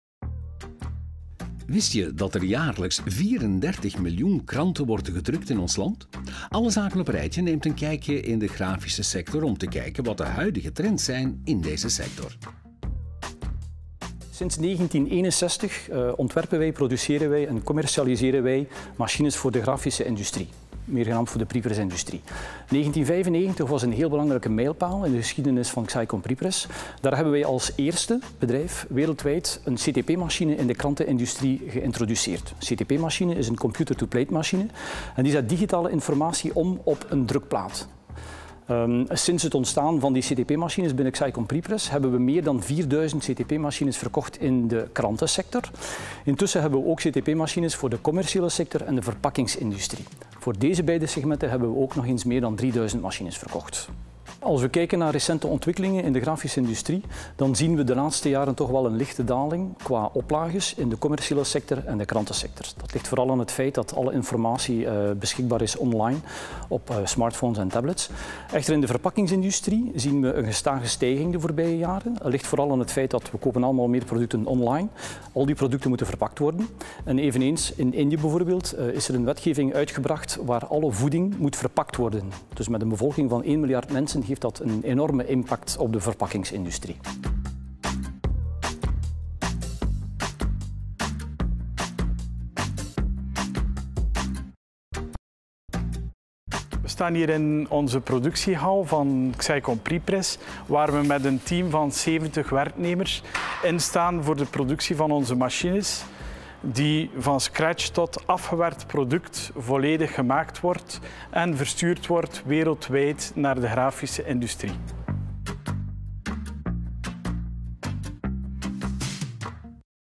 Xeikon
Wist je dat er jaarlijks 34 miljoen kranten worden gedrukt in ons land? Alle Zaken Op Een Rijtje neemt een kijkje in de grafische sector om te kijken wat de huidige trends zijn in de sector! Wij gingen langs bij Xeikon.